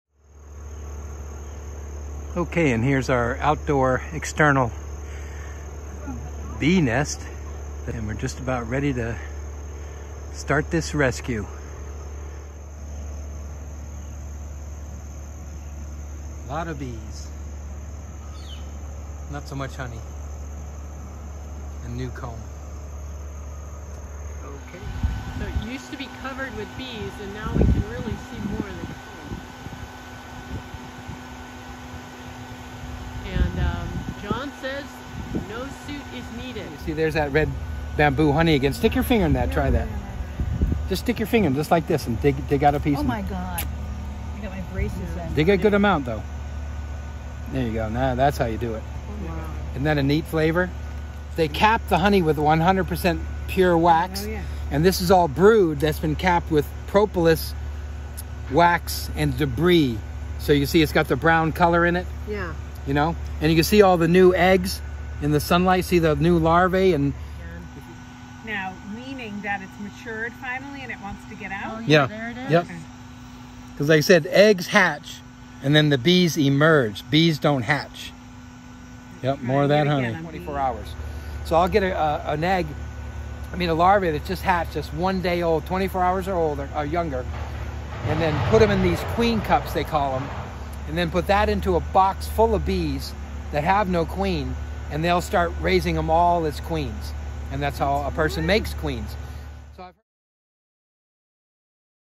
Beehive Rescue on Hemlock Drive
Hemlock Drive homeowners call on Jon Nelson of B B Nelson Apiaries to remove a honeybee hive after it was discovered on the dead limb of a tree they wanted to remove. September 2023